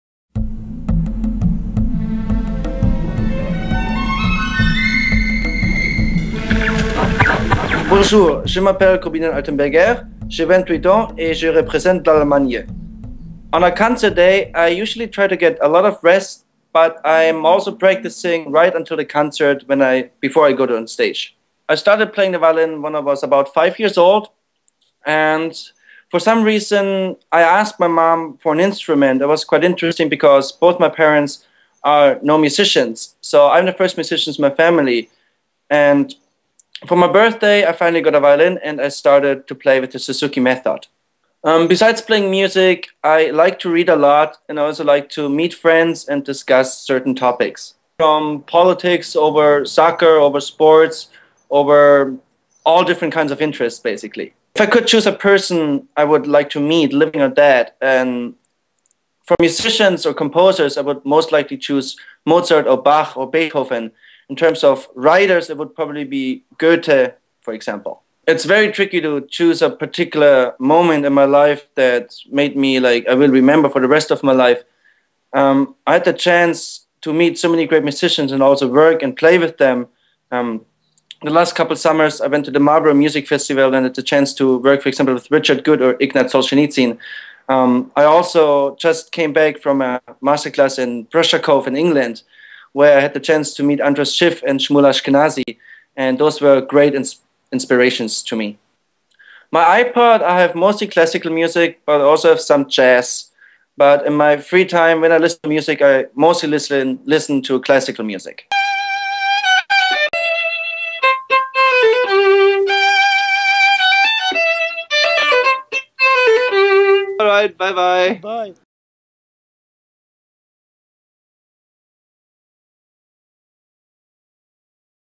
Concours musical international de Montréal - CMIM violin (2010) - Korbinian Altenberger
Over one hundred candidates, among the finest young violinists in the world, registered for the preliminary round of the CMIM 2010. 24 of them—9 women and 15 men—are moving on to the following rounds, which will take place from May 25 to June 4. Countries with the greatest number of participants are the United States (5 candidates) and South Korea (4). Next comes Germany (3); Japan (3), China (2), Canada (2) and Russia (2); and France, Poland, and the Czech Republic (1).